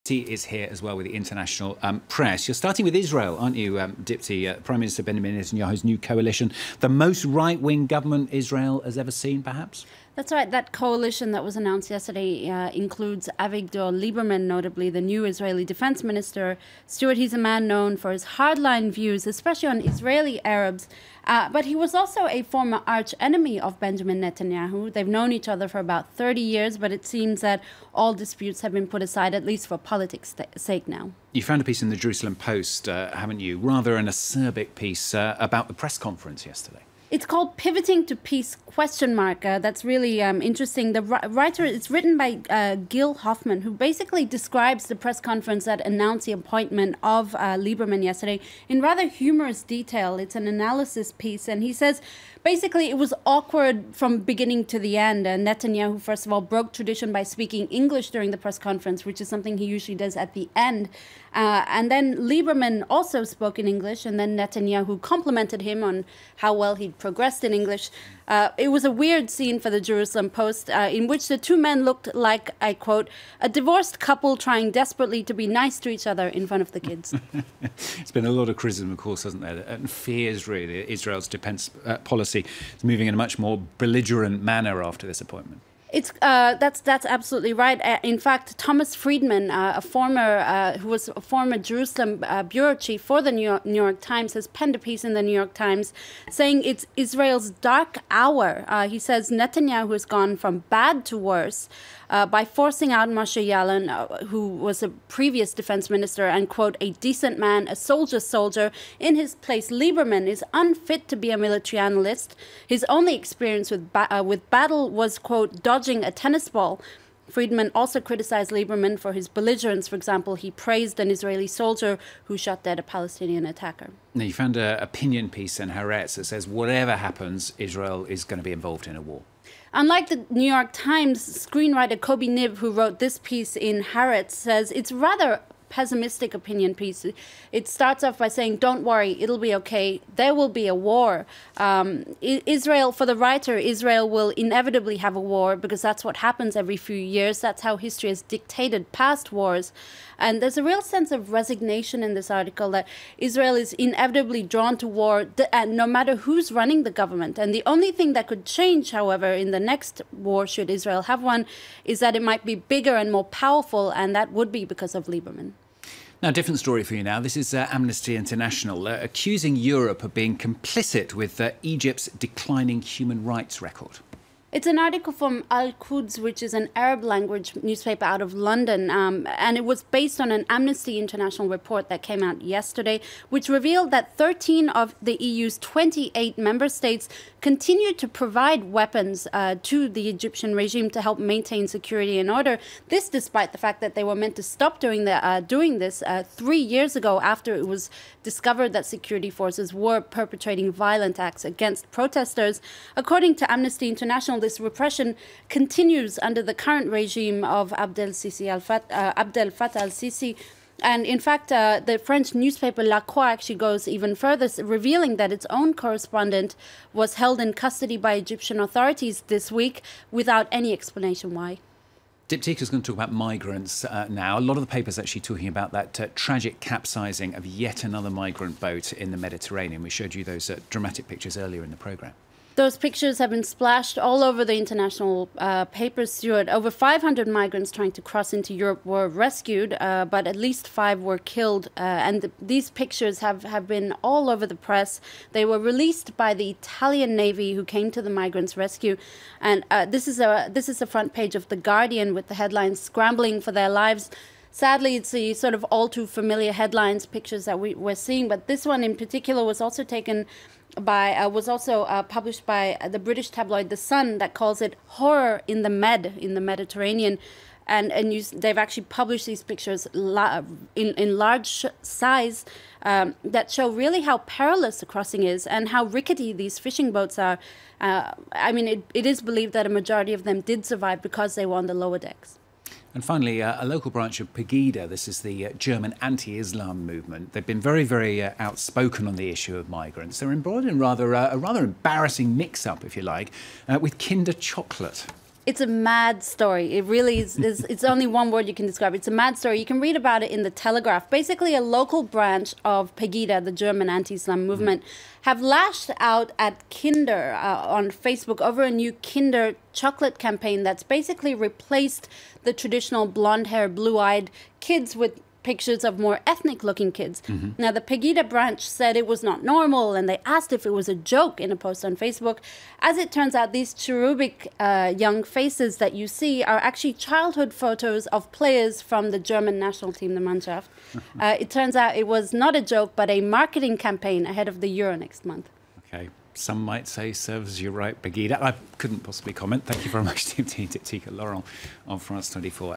Netanyahu's coalition: 'Israel's dark hour'
INTERNATIONAL PAPERS - Thursday, May 26: There are a lot of opinion pieces on Israeli Prime Minister Benjamin Netanyahu's new coalition. Meanwhile, Amnesty International accuses the EU of being complicit with the Egyptian regime. Also, many newspapers publish the shocking photos of a migrant boat collapsing in the Mediterranean and the German anti-Islam movement Pegida finds itself in an embarrassing mix-up with... Kinder chocolate.